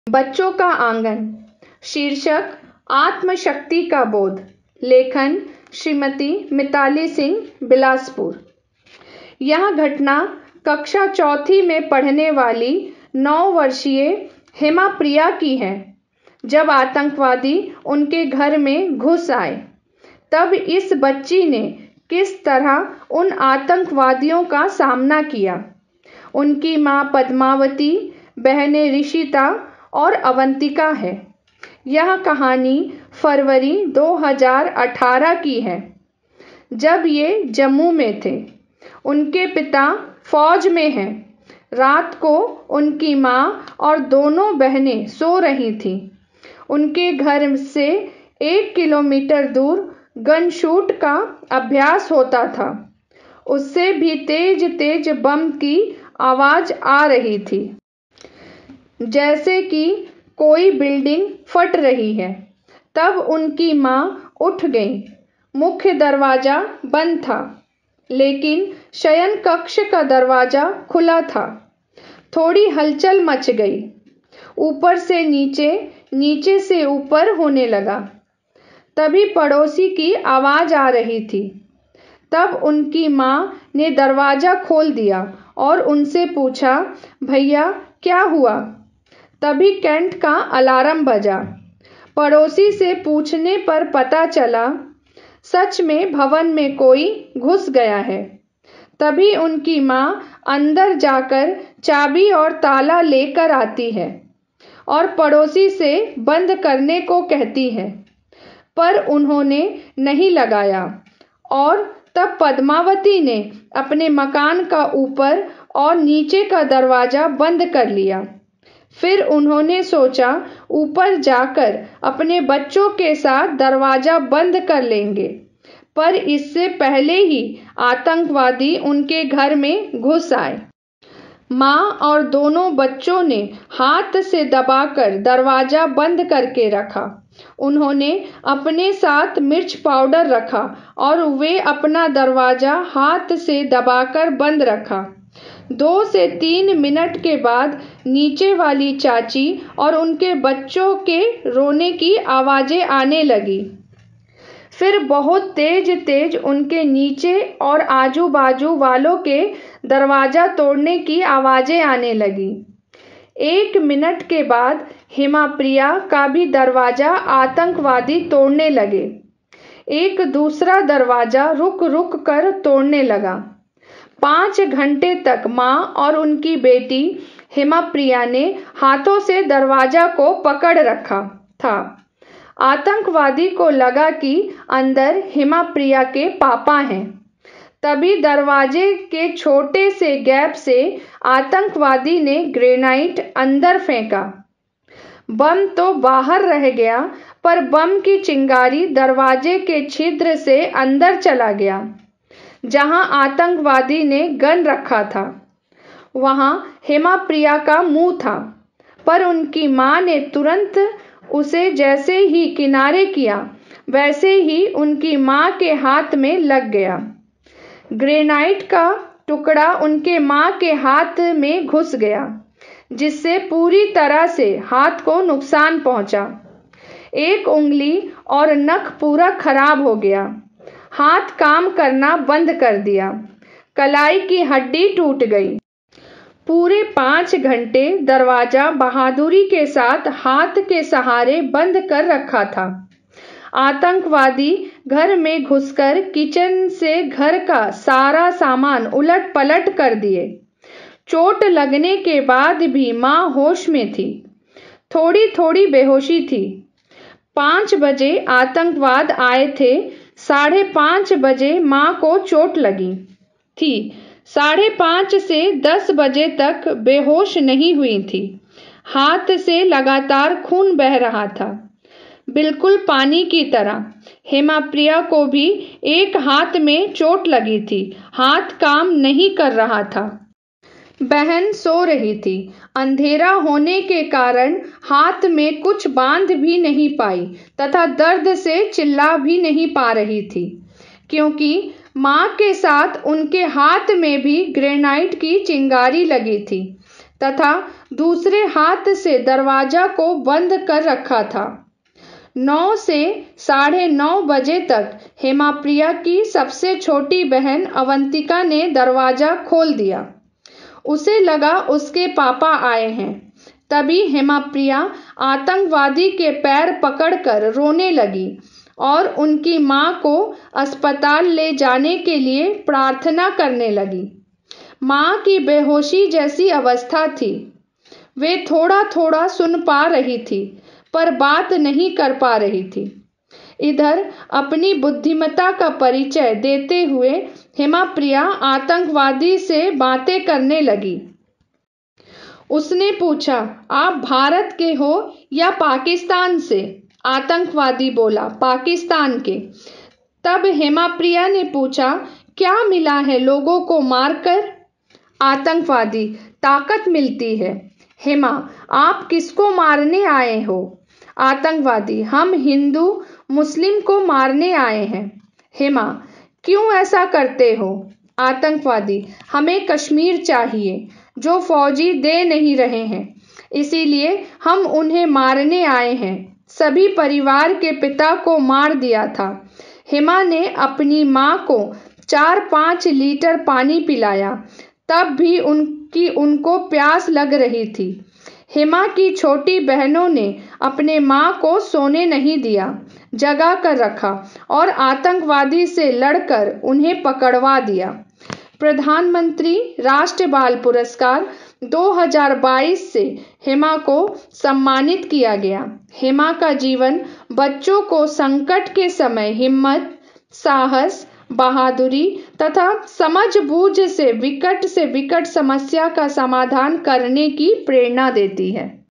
आत्मशक्ति का बोध.लेखक और स्वर-श्रीमती मिताली सिंह,बिलासपुर.फरवरी-23,पृ.141
VIVEK JYOTI HINDI MONTHLY MAGAZINE Published by RAMAKRISHNA MISSION VIVEKANANDA ASHRAMA, Raipur, Chhatishgarh-492001.